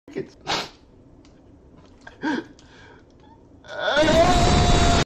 Literally Just Jschlatt Crying In A Pope Hat
that's it. literally that's all this video is why are you reading the description?